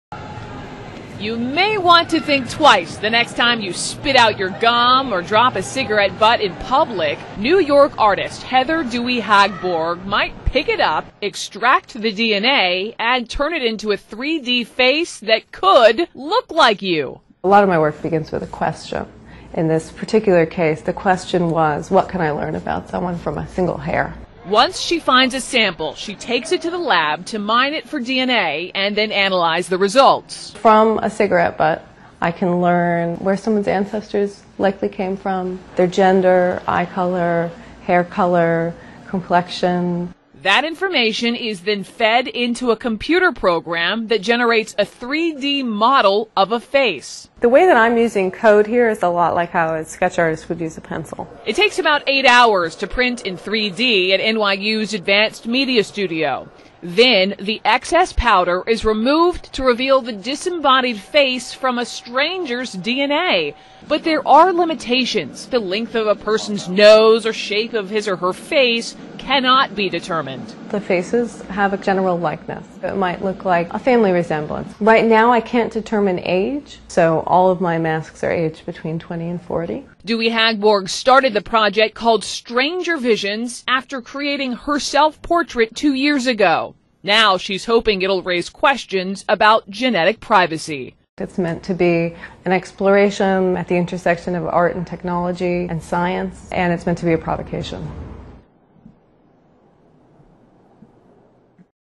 Making 3-D faces from stranger's DNA
A New York artist can tell a lot about you from information you're unknowingly leaving behind.

These disembodied faces are created by a New York artist using the DNA of strangers and a 3-D printer -- it offers a good, though not perfect, idea of what the person may have looked like.

Heather Dewey-Hagborg mines the DNA from sources such as discarded cigarette butts, gum and stray hairs -- the genetic information is then fed into a computer program to create a virtual model.

A 3-D printer does the rest.

She started the project with her own DNA. She hopes the project will raise questions about genetic privacy.